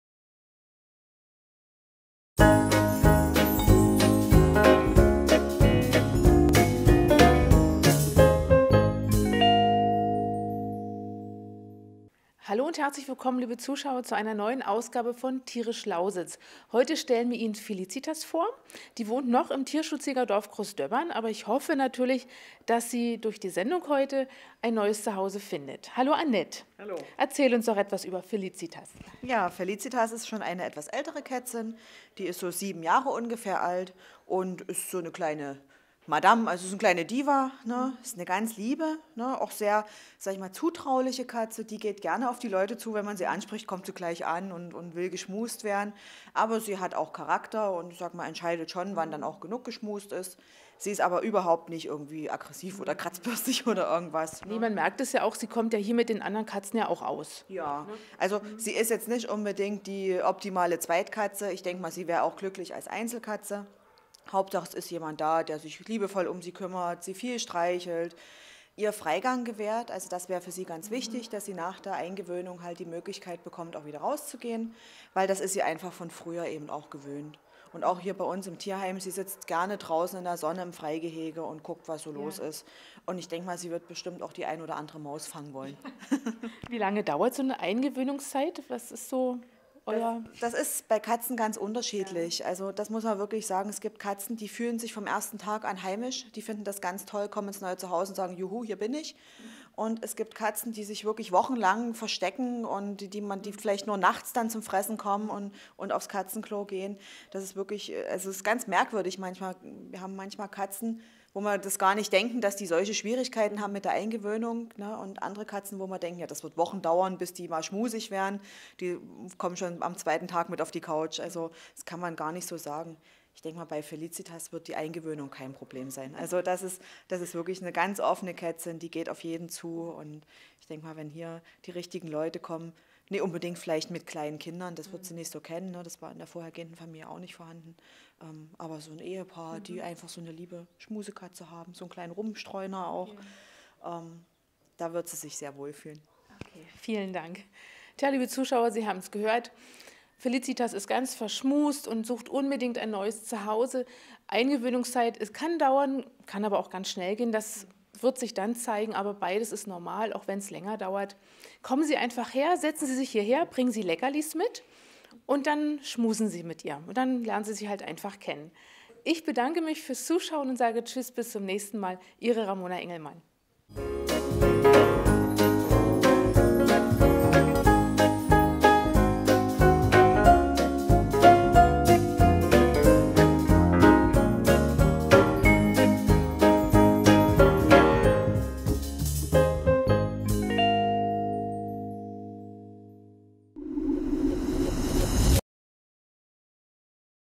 Felicitas | Tierisch Lausitz
Hauskatze Felicitas sucht dringend eine neues Zuhause.  Sie ist eine liebe, unkomplizierte und umgängliche Kätzin. Wir wünschen uns für sie eine Familie, wo sie geliebt und umsorgt wird und die Möglichkeit zum Freilauf bekommt.